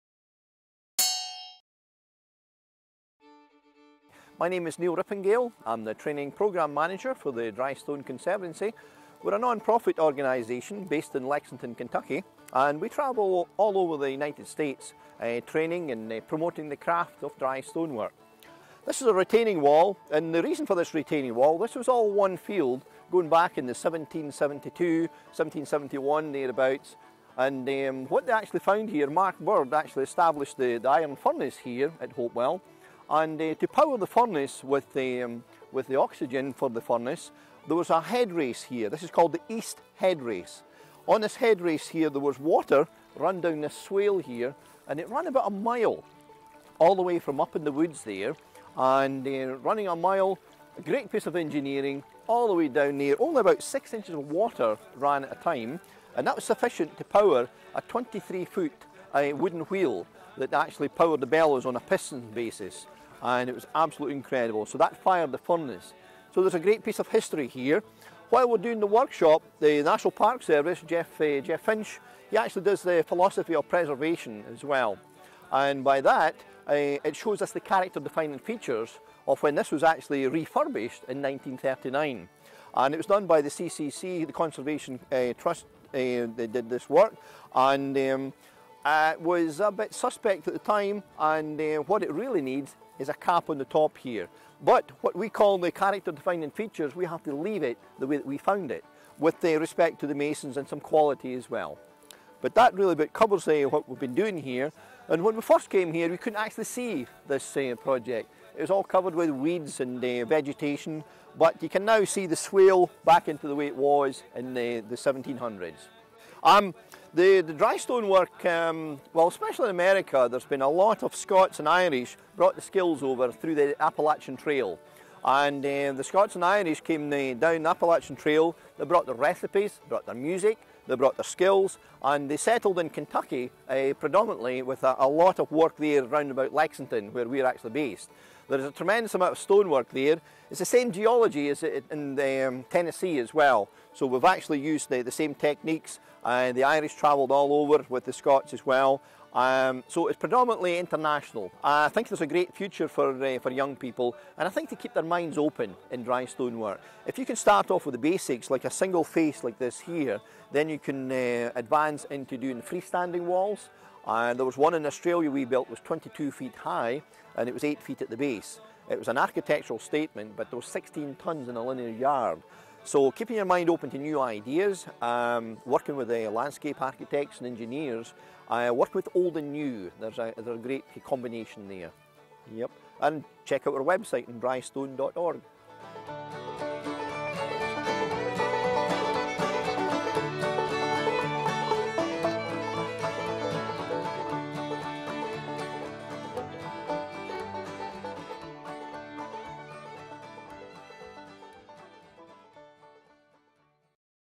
Dry Stone Wall Construction
Dry stacked stone expert, Neil Rippingale discusses the efforts of the Dry Stone Conservancy to restore a historic stone wall at Hopewell Furnace in Pennsylvania.

Transcript:
My name is Neil Rippingale, I'm the training program manager for the Dry Stone Conservancy. We're a nonprofit organization based in Lexington Kentucky and we travel all over the United States training and promoting the craft of dry stonework. This is a retaining wall and the reason for this retaining wall, this was all one field going back in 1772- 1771 there about. What they actually found here Mark Bird established the iron furnace here at Hopewell and to power the furnace with oxygen for the furnace, there was a headrace here this is called the east headrace. On this headrace here there was water running down this swell here and it ran about a mile. All the way from up in the woods there, and running a mile. A great piece of engineering, all the way down there all about six inches of water ran at a time and that was sufficient to power a twenty three foot wooden wheel that actually powered the ballers on a piston basis and it was absolutely incredible so that fired the furnace. So there is a great piece of history here. While we're doing the workshop, the National Park Service Jeff Finch actually does the philosophy of preservation as well and by that it shows us the character defining features of when this was actually refurbished in 1939. It was done by the CCC, the Conservation Trust did this work and it was a bit suspect at the time and what it really needs is a cap on the top here. But what we call the character defining features, we have to leave it the way that we found it with respect to the masons and some quality as well. But really covers what we've been doing here and when we first came here we couldn't actually see this project it was all covered with weeds and vegetation. But you can now see the swell back the way it was back in the 1700s. The dry stone work especially in America there's been a lot of Scots and Irish who brought the skills over through the Appalachian Trail. The Scots and the Irish came down the Appalachian Trail, the brought their recipes, they brought their music, they brought their skills, and they settled in Kentucky predominately with a lot of work there around Lexington where we're actually based. There's a tremendous amount of stonework there, it's the same geology in Tennessee as well so we've actually used the same techniques the Irish traveled all over with the Scots as well. So it's predominately international. I think there's a great future for young people and I think  they should keep their minds open to dry stone work. If you can start off with the basics, with a single face like this wall here then you can advance into doing freestanding walls. There's one in Australia we built was twenty two feet high and it was eight feet at the base. It was an architectural statement but there was sixteen tons in a linear yard. So keeping your mind open to new ideas, working with landscape architects and engineers. Work with old and new, they're a great combination there.